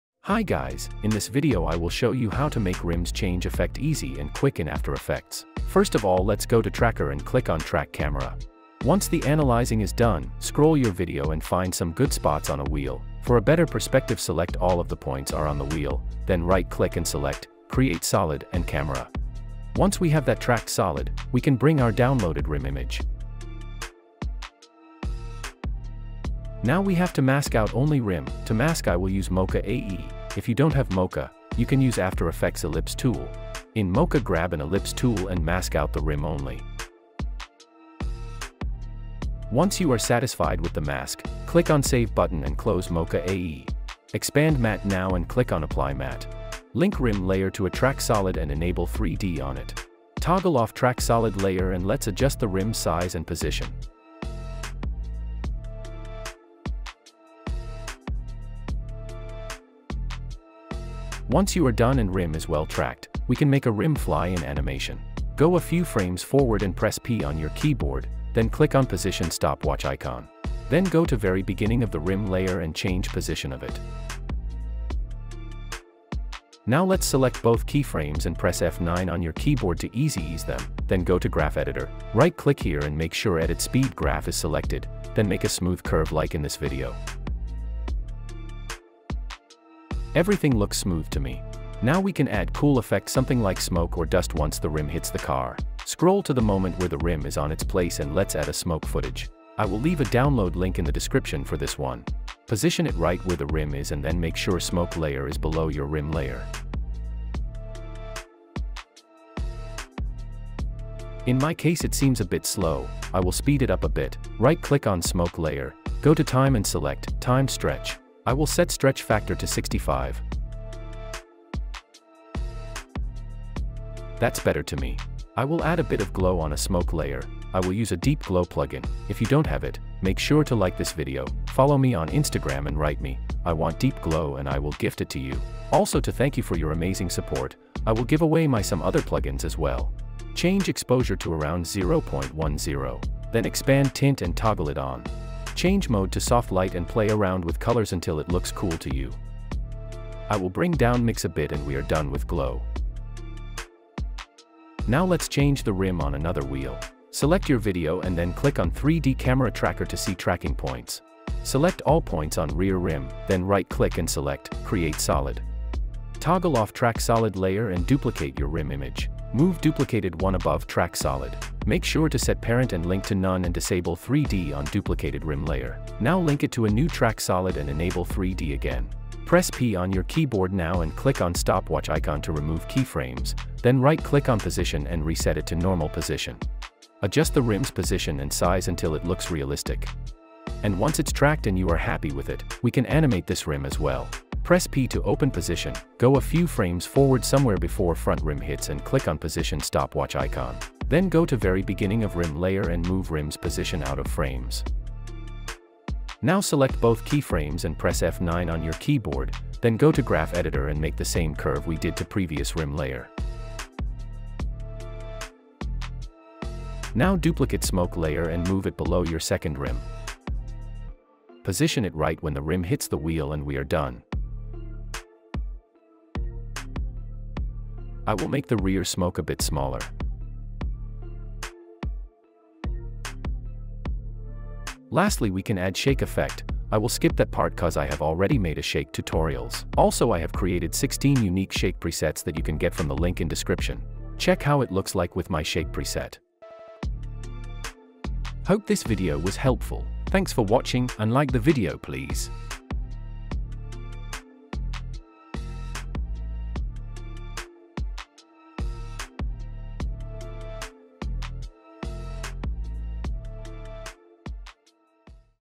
Car Wheel Changing Effect in After Effects
In this tutorial I will show you how to make a wheel changing effect in Adobe After Effects

CPU: i7-4790
GPU: GeForce RTX3050 VENTUS 8GB
RAM: 16 GB